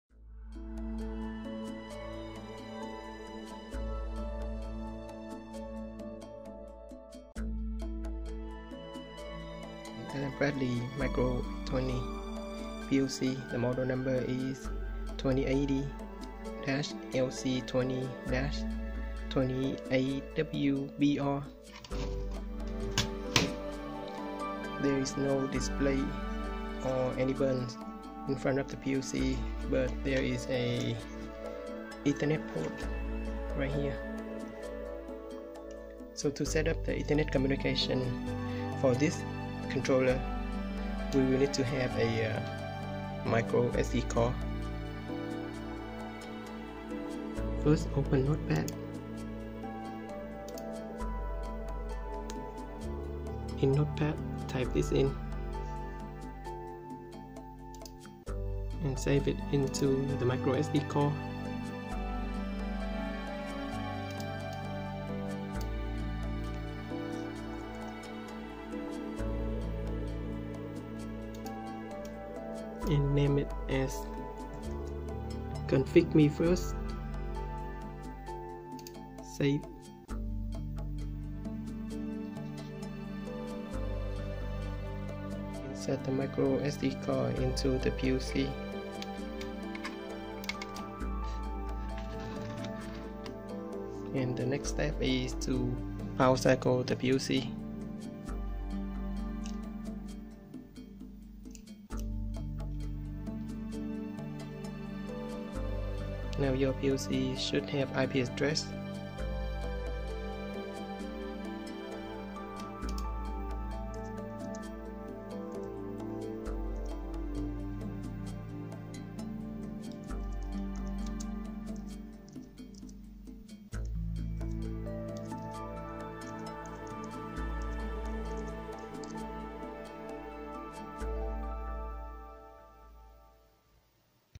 Micro820PLC Ethernet Communication IP Address Setup/MicroSD
NOTE: After IP address is successfully assigned to PLC, turn it off then remove the microSD card.

[PM] Powerup: Power up and switch to PROGRAM mode.
[CF] Powerup: Power up and attempt to clear fault.
[IPA = xxx.xxx.xxx.xxx] Powerup: Power up and set IP address to xxx (must be numbers only).
[SNM = xxx.xxx.xxx.xxx] Powerup: Power up and set subnet mask to xxx (must be numbers only).

Rockwell AB
Micro820
Ethernet Communication Setup with micro SD card

Sample Code:
[PM]
[CF]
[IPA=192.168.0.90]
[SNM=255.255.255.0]
[END]